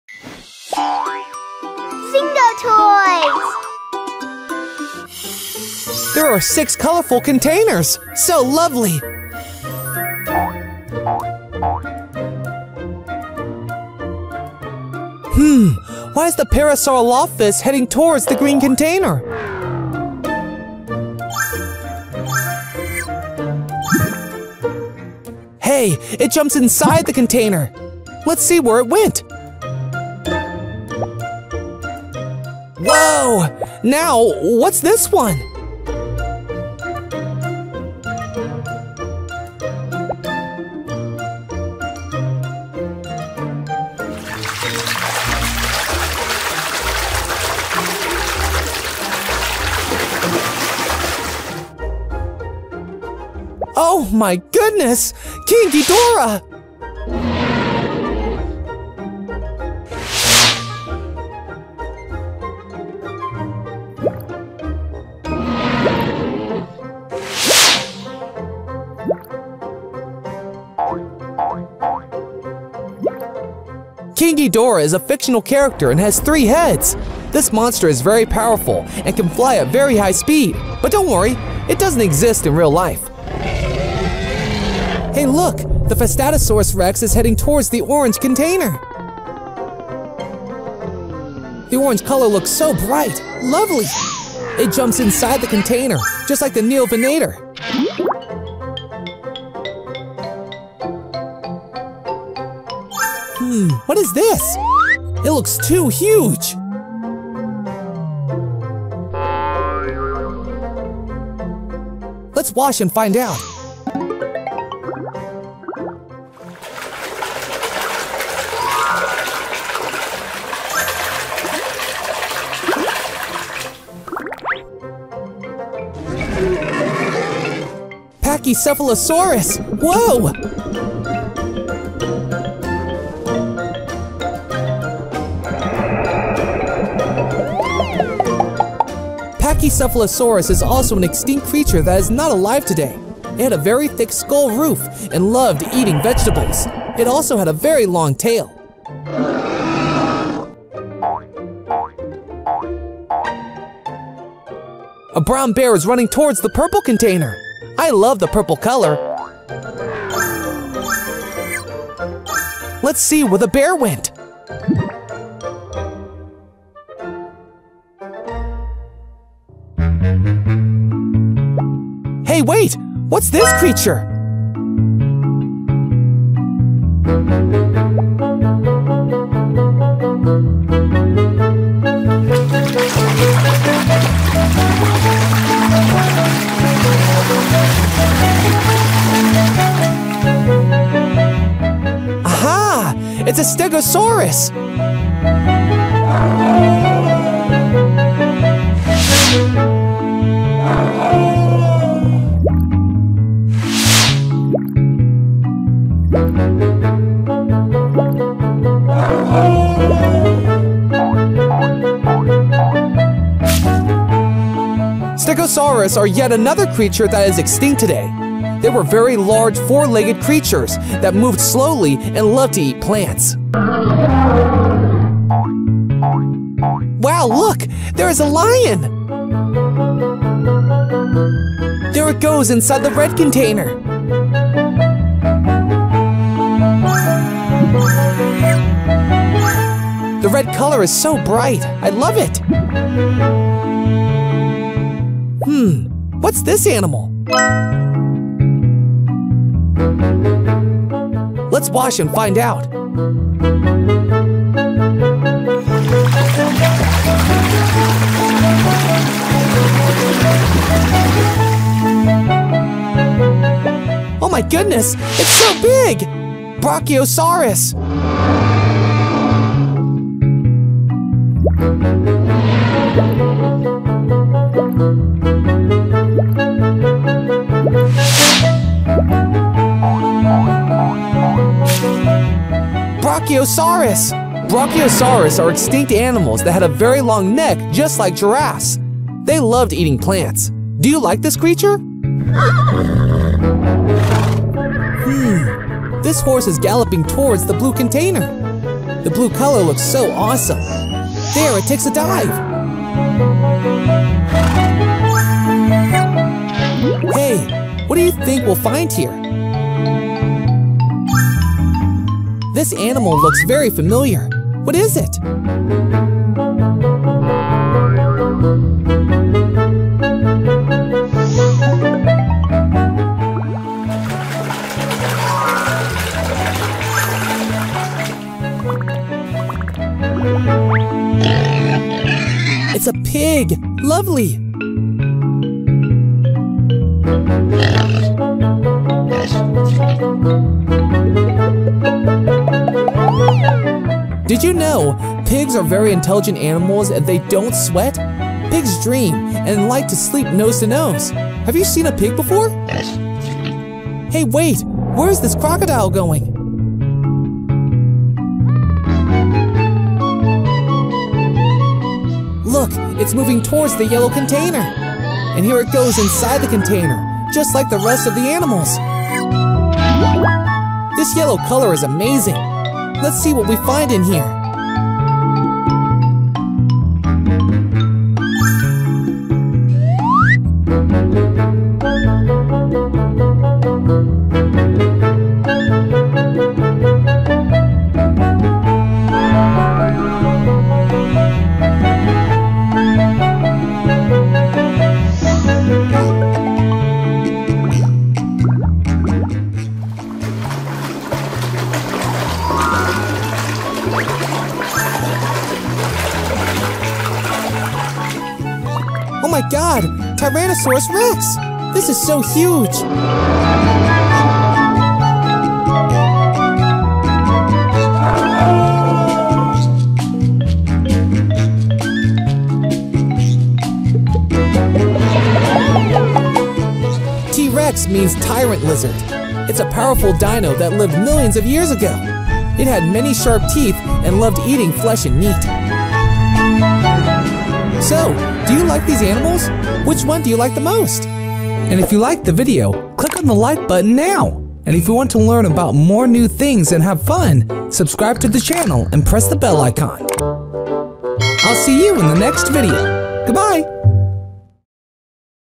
Wild Animals Become Mystery Creatures After Jumping in Magic Balls | Fun Video for Kids
Dinosaurs and wild animals are jumping into the colorful balls and transforming. Learn about animals like King Ghidorah, T-Rex, Pachycephalosaurus, Stegosaurus, Brachiosaurus and Pig while having fun. Learn about colors and animals in this educational video.